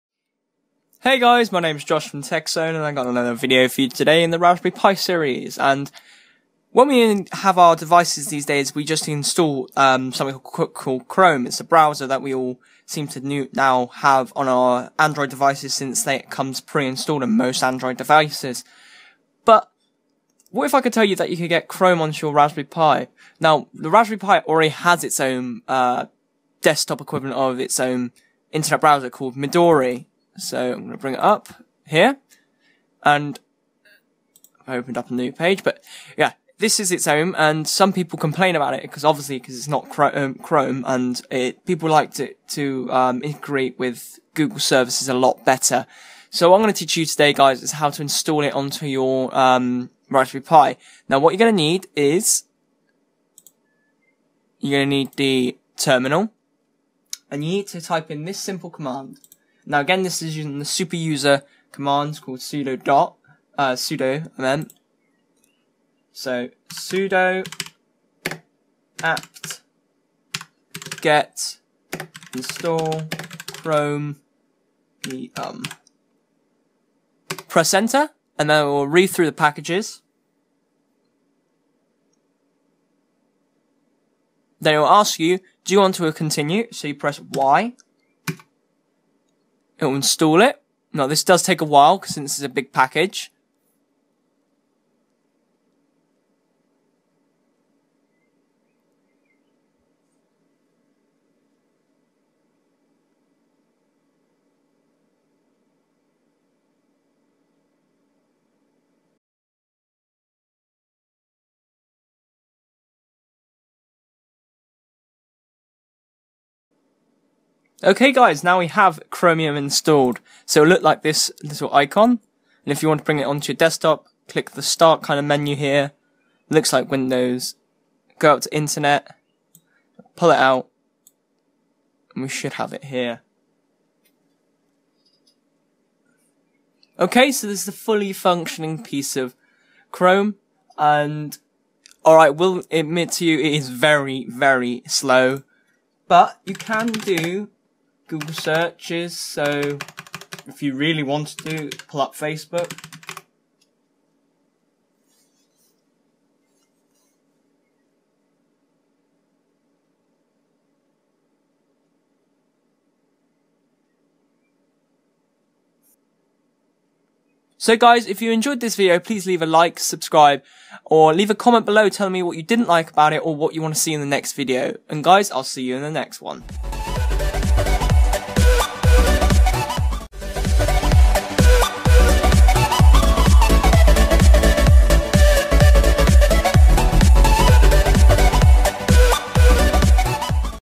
how to install Google Chrome (chromium) on the Raspberry pi
want chrome on your raspberry pi look no further !!!